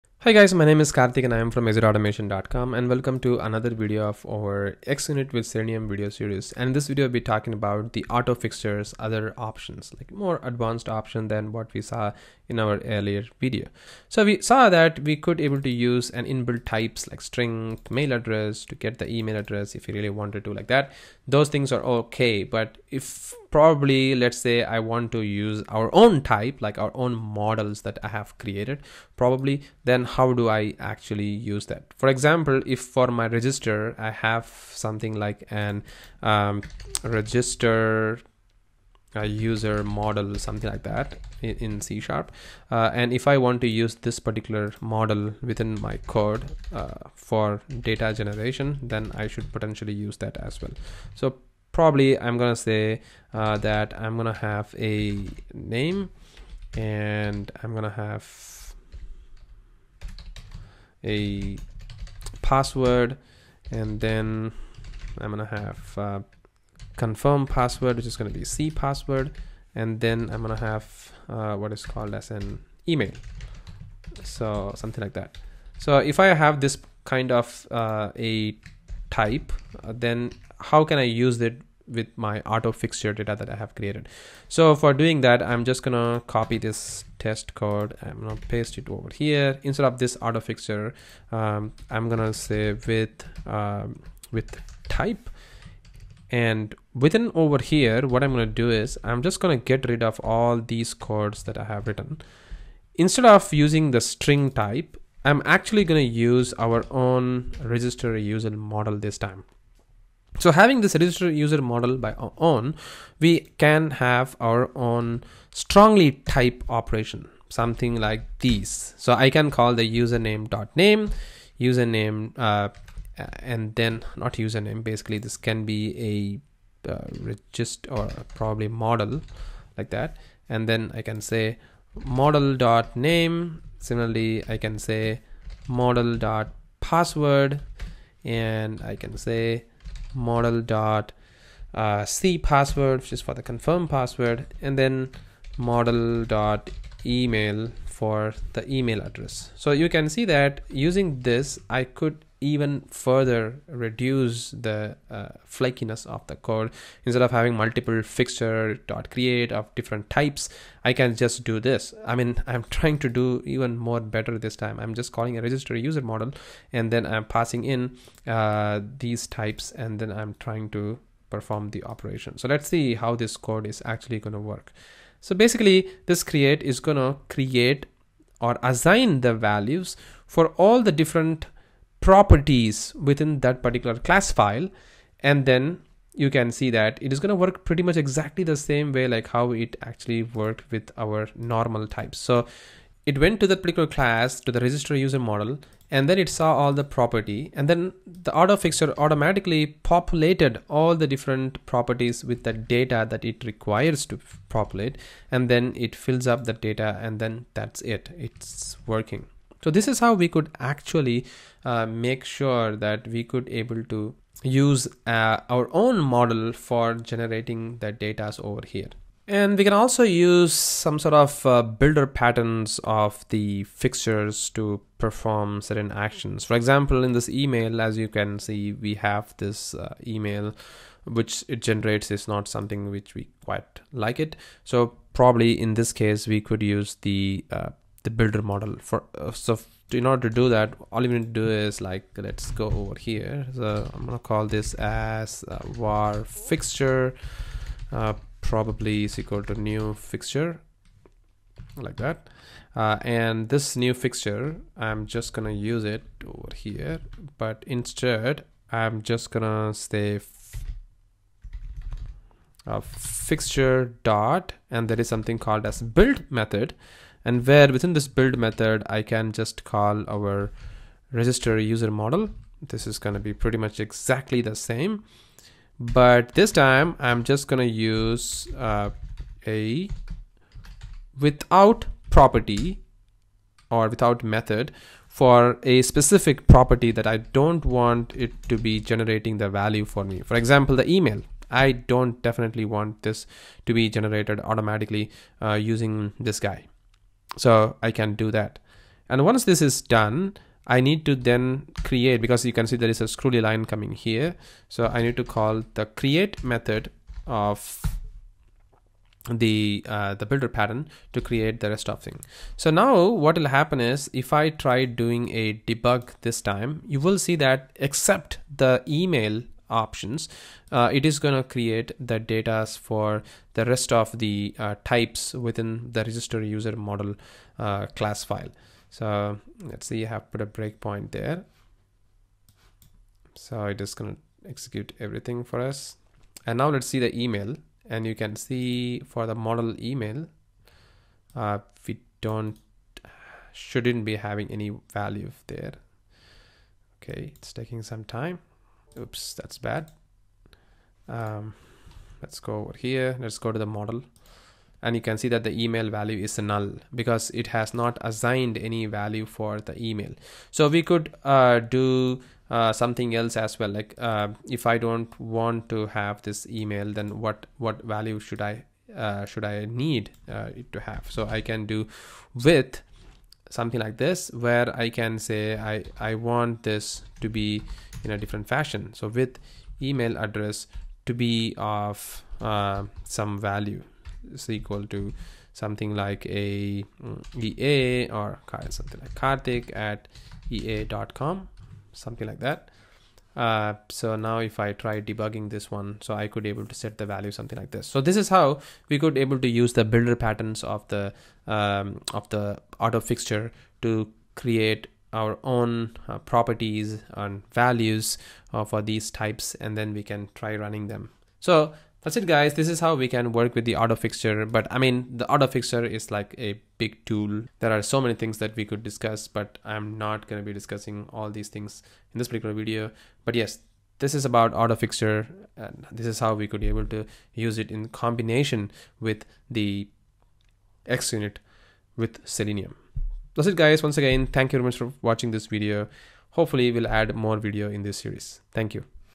Part 9 - Customized Test Data creation using Builder Pattern of AutoFixture
In this video, we will discuss Customized Test Data creation using Builder Pattern of AutoFixture.

AutoFixture is a tool designed to make Test-Driven Development more productive and unit tests more refactoring-safe. It does so by removing the need for hand-coding anonymous variables as part of a test’s Arrange phase